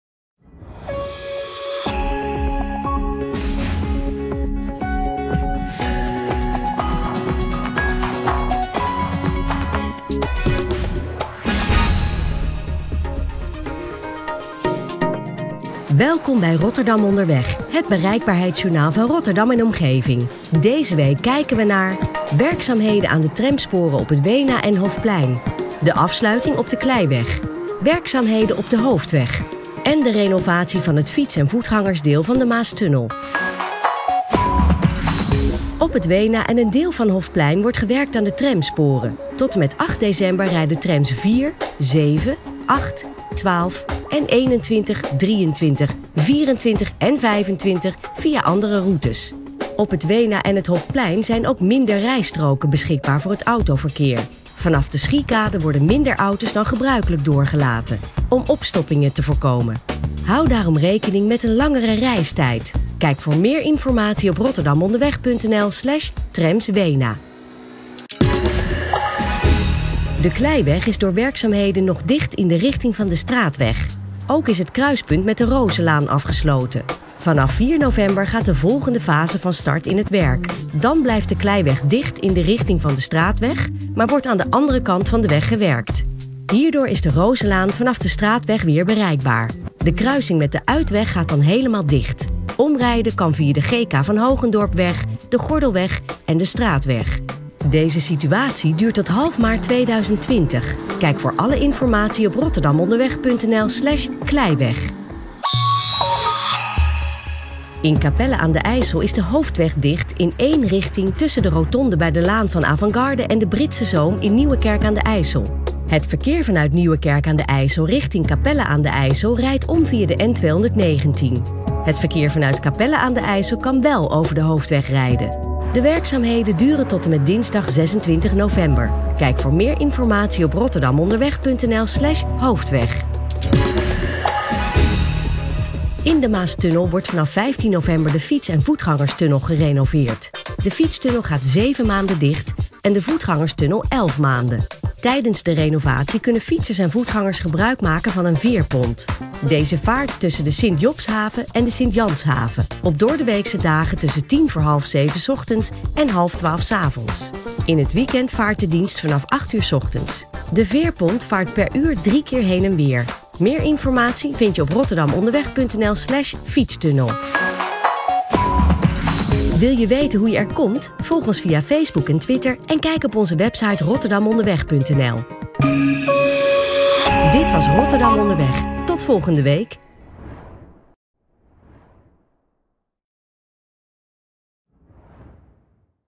Rotterdam Onderweg
Deze week kijken we in Rotterdam Onderweg naar: werkzaamheden aan de tramsporen op het Weena en Hofplein. De afsluiting op de Kleiweg. Werkzaamheden op de Hoofdweg. En de renovatie van het fiets- en voetgangersdeel van de Maastunnel.